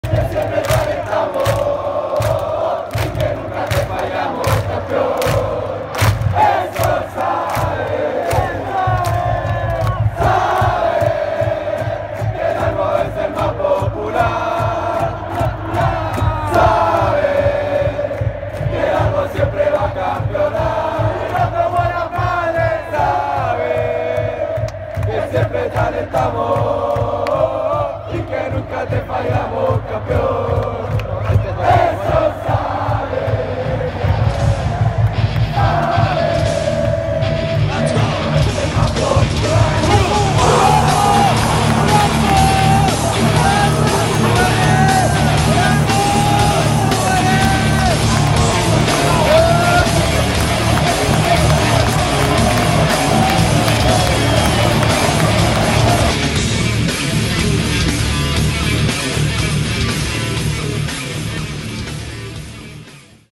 Golazo Gabriel Costa a Santiago Wanderers
Golazo de Costa a los Caturros, partido válido por la fecha 30 del Campeonato Planvital 2021